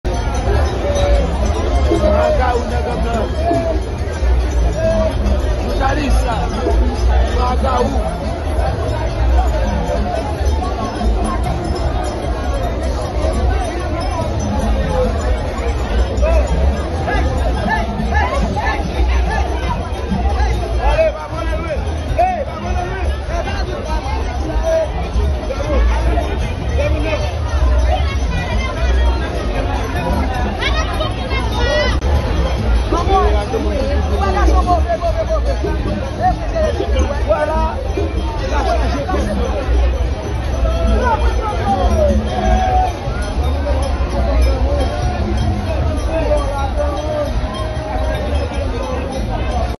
Mezanmi wap sezi wè kijan Legba Plonje nan Basen St Jacques nan okazyon fèt Pleine du nord
wap sezi wè jan Legba Plonje nan Basen St Jacques
Haiti Media One
Echo Lakay

Jouna9#HaitianNews# LiveJounal#LiveNews